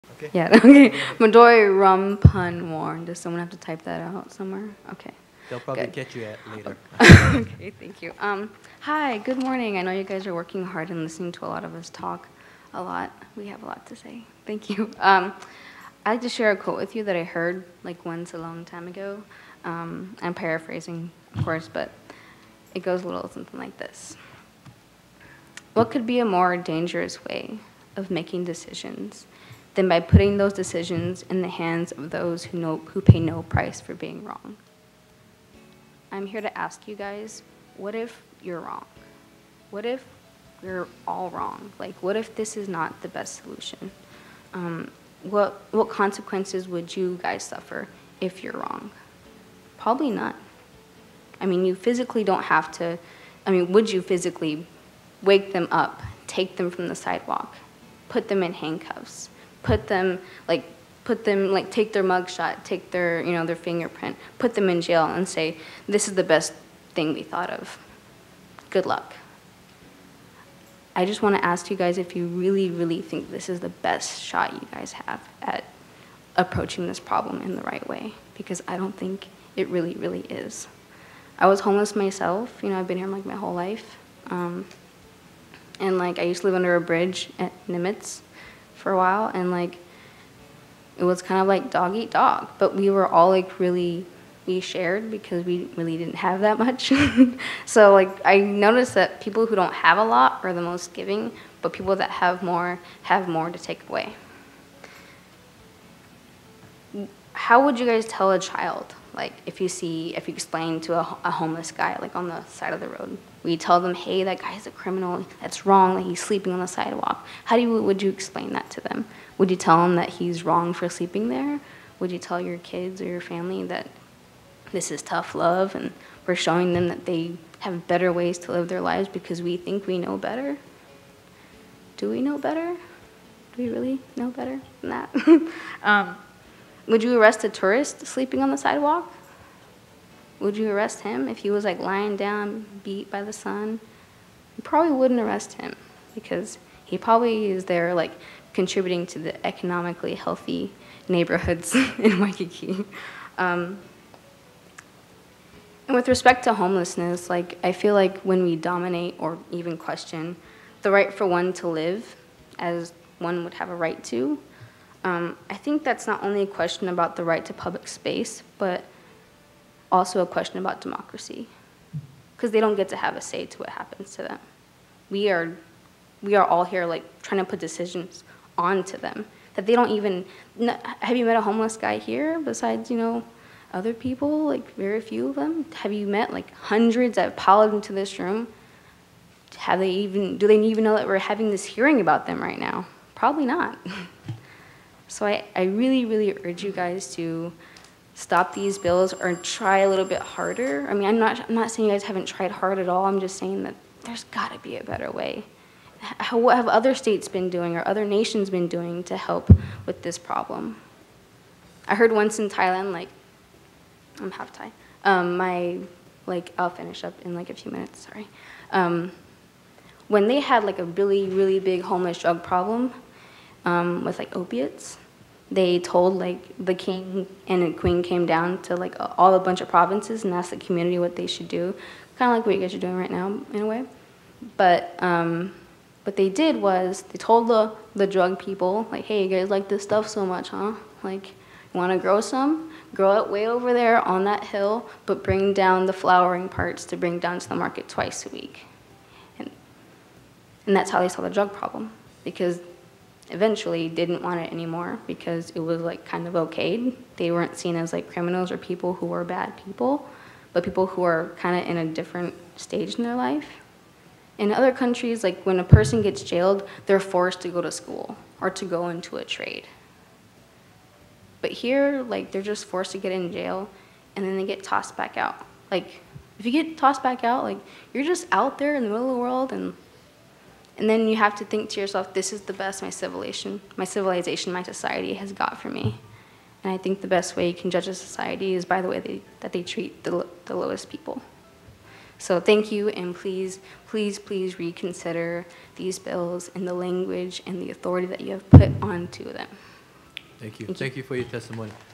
7/9/14 Madori Rumpungworn's testimony against criminalizing homeless
From the 7/9/2014 hearing on Bills 42 43, 44, 45, 46, and 48 criminalizing homelessness. Madori Rumpungworn's testimony opposing.